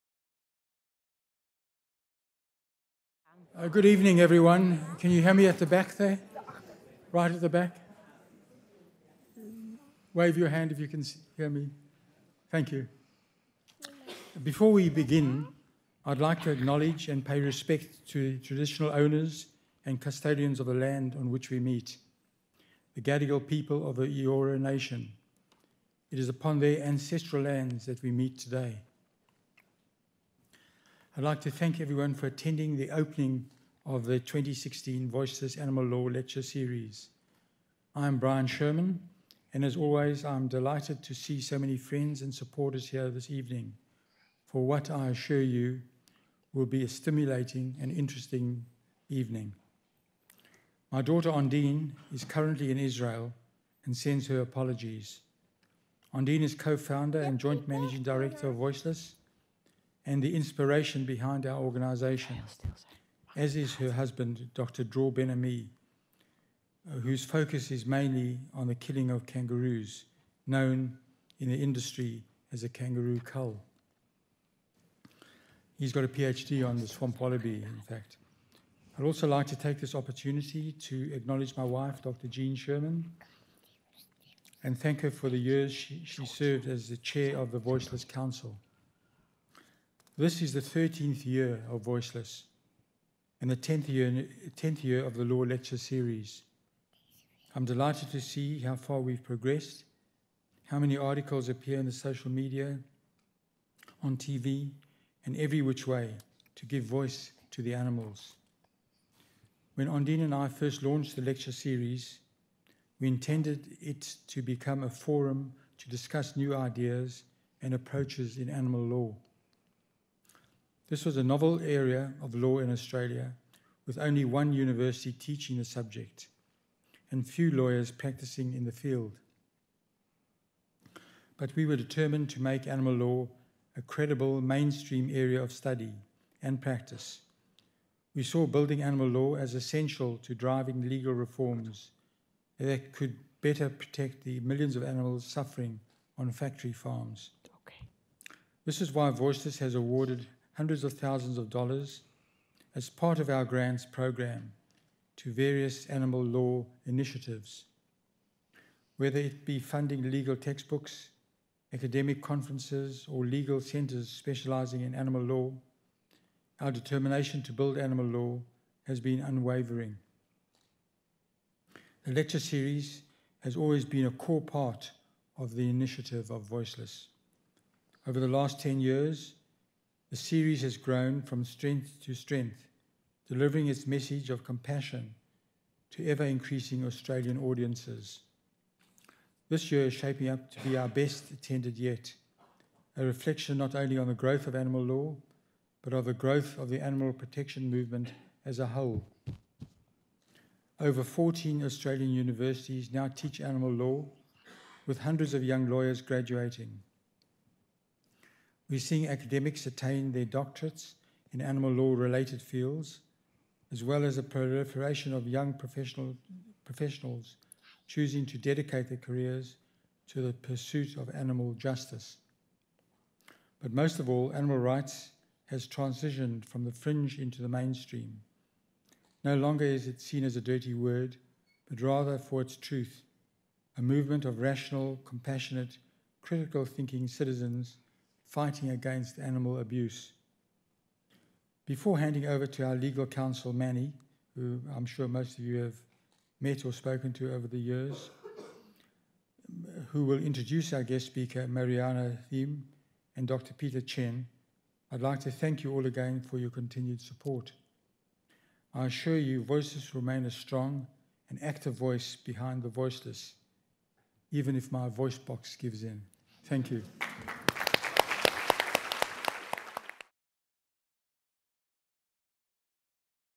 Brian Sherman AM - Welcome
2016 Voiceless Animal Law Lecture Series - Political Animals

Watch Voiceless Co-Founder Brian Sherman AM open the 2016 Voiceless Animal Law Lecture Series.

This Federal Election year, the Series looked at the rapidly growing global movement to represent animals in politics, and use political processes to achieve positive change for animals. The keynote address was delivered by prominent voice and pioneer of this political movement, Marianne Thieme.

This video was recorded at the Keynote Lecture held at the University of Sydney on Wednesday 4 May 2016 and comes courtesy of Outshine Media.